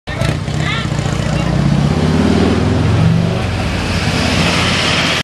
Zombie Pub Crawl 2010 - VID00144.mp4
- Minneapolis Zombie Pub Crawl 2010 - Taken in front of Maxwell's on Washington Ave S in the Mill District of Minneapolis. ZPC6